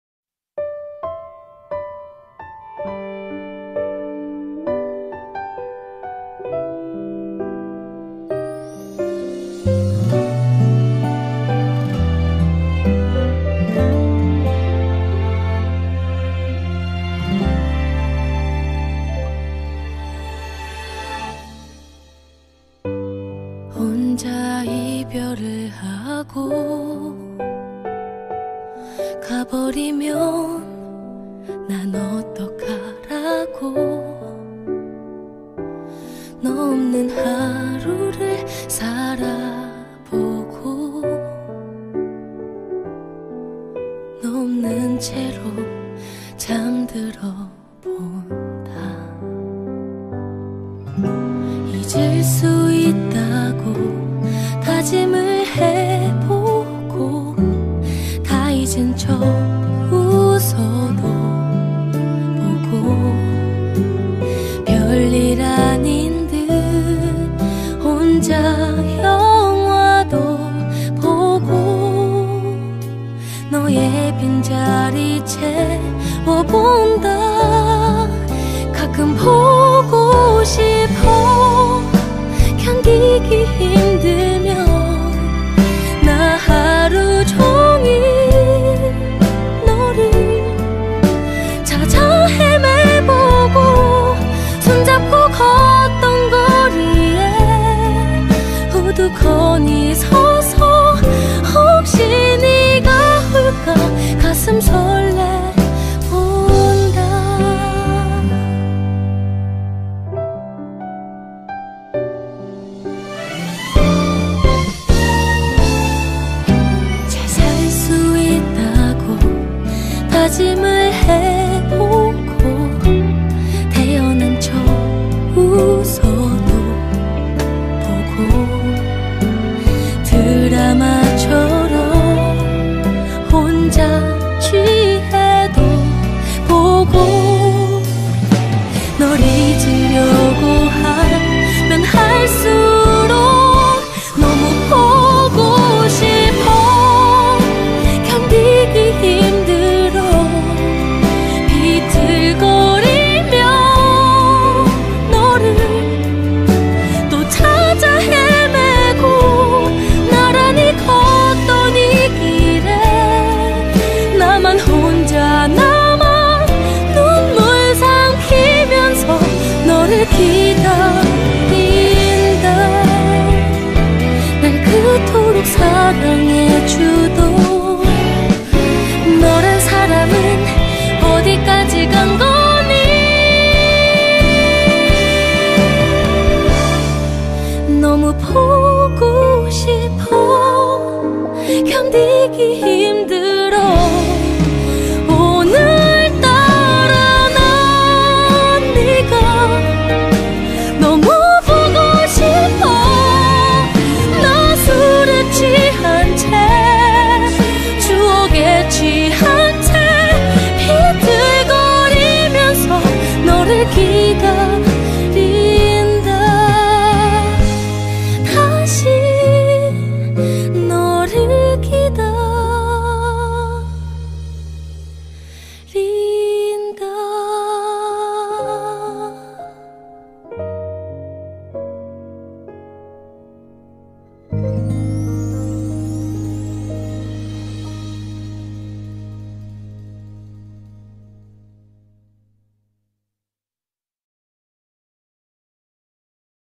손잡고 걷던 거리에 우두커니 서서(가평 명지산)
파아란알파인클럽 제14차 정기산행
가평 명지산 29명 참석(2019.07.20 토)
참석하신 모든 분들께 감사드립니다.
선두(언경대장) 중간(용성대장) 후미(주석대장)